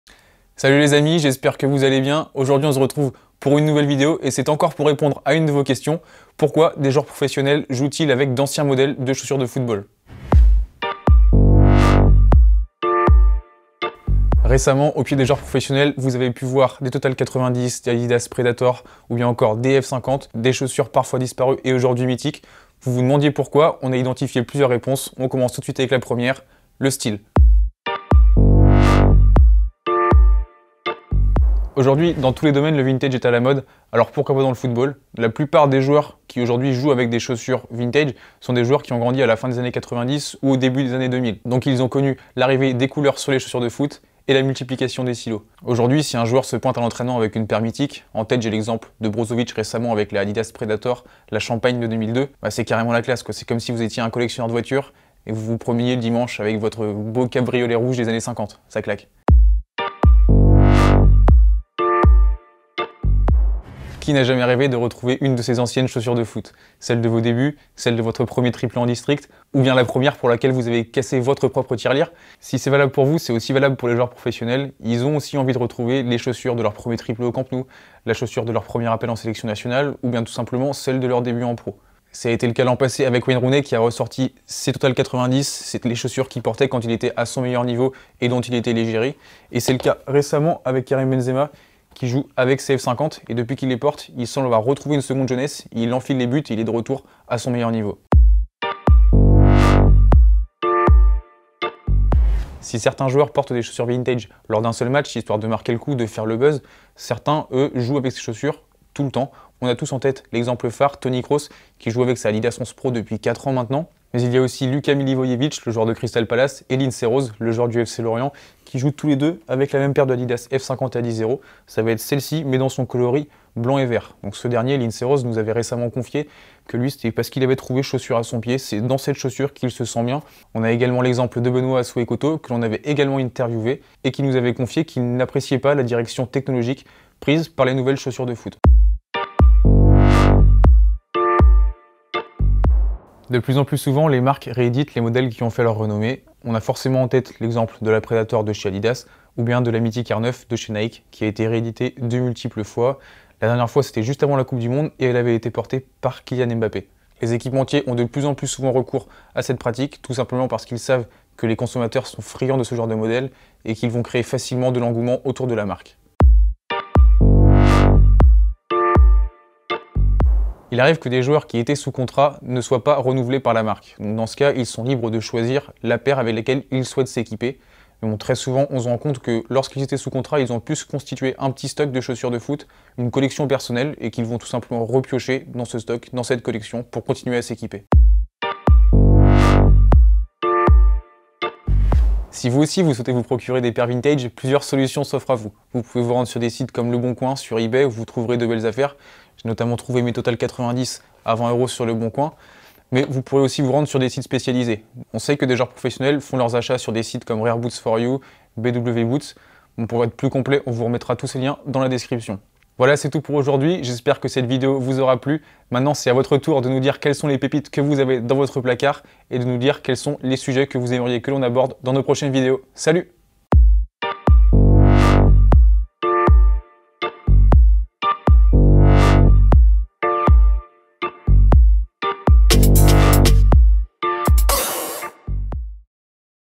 Pourquoi des pros jouent avec des chaussures anciennes ou vintage ?
Benzema en adidas F50, Rooney en Total 90, Lacazette en Tiempo R10.. De plus en plus les joueurs ressortent des anciens modèles de chaussures de foot. Vous avez été nombreux à nous demander pourquoi, on a essayé d'y répondre.